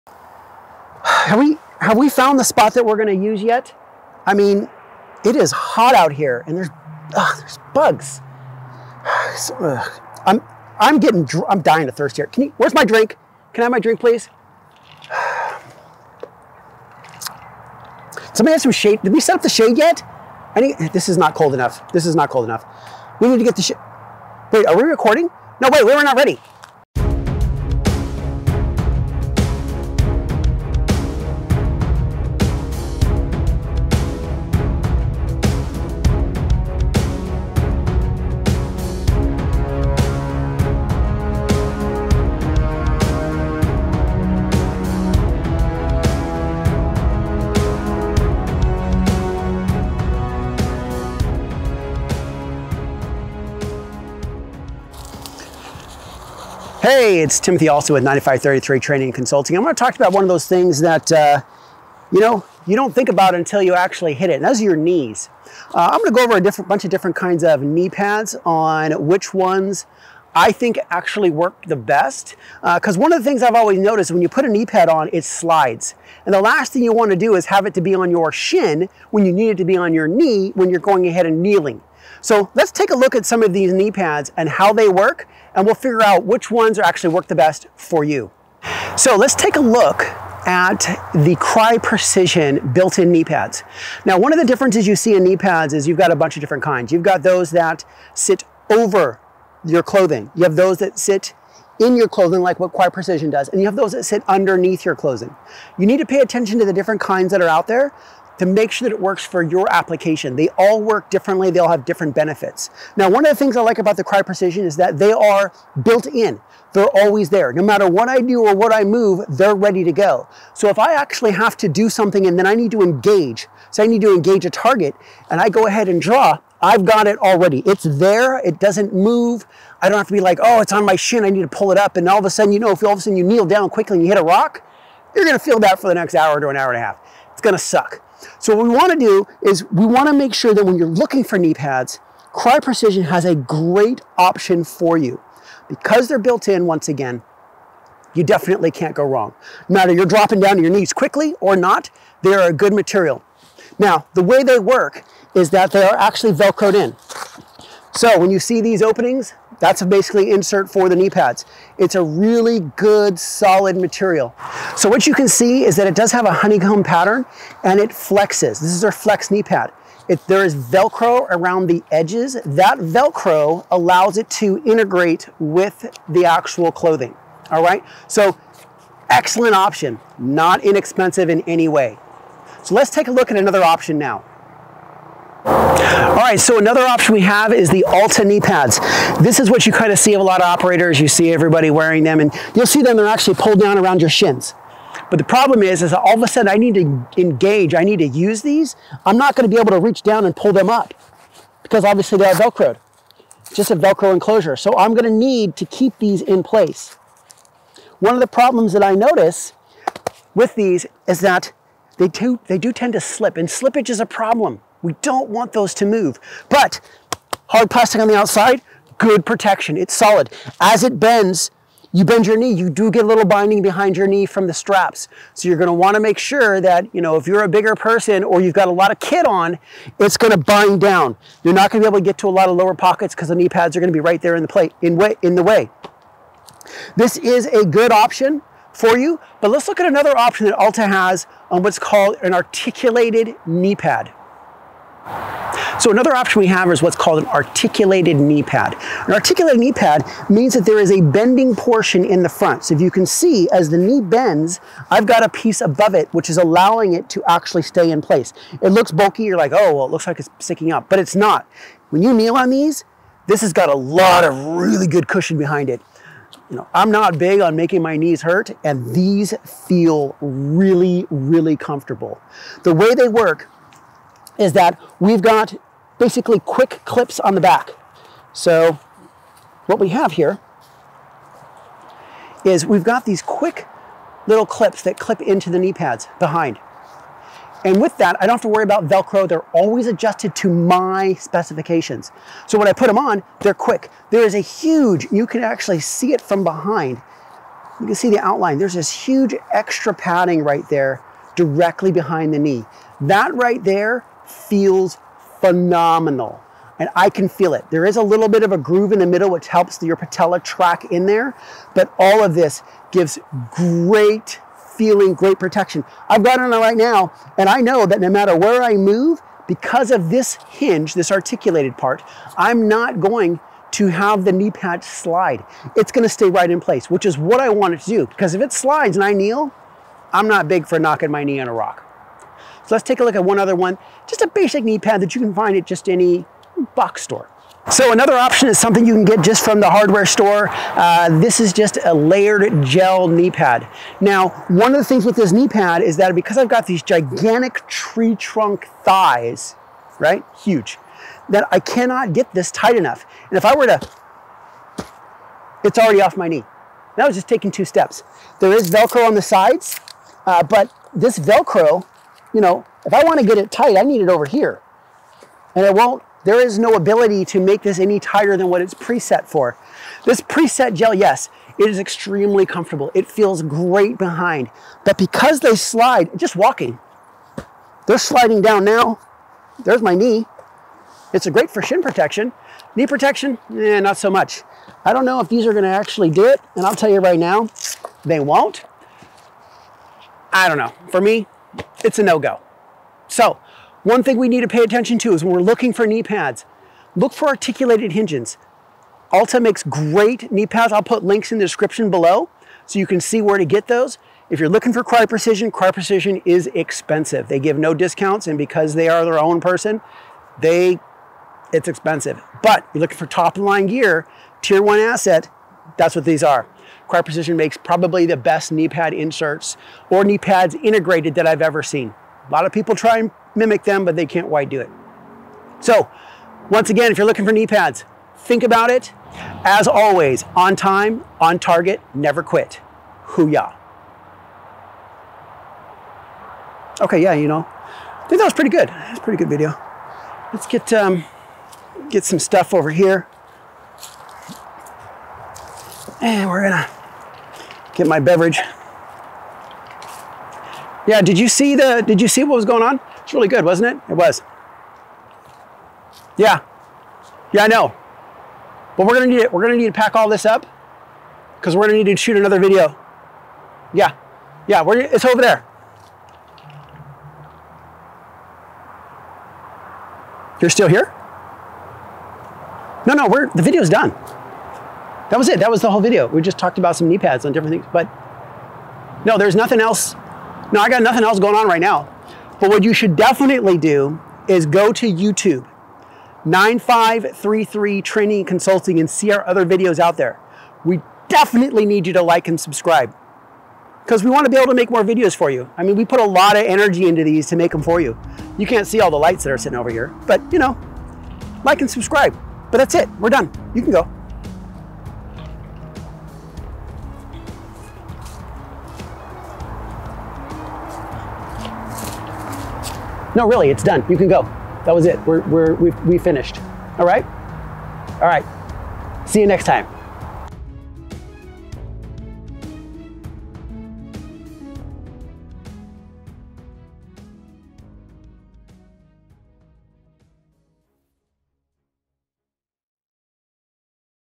combat knee pads Kneepads, Do You Need Them? - US Navy SWCC
combat knee pads are important for protection and agility during combat, allowing you to fall in stable positions as fast and safe as possible.

Check this week's episode, we try out many brands and types of Knepads to recommend you the best for your situation.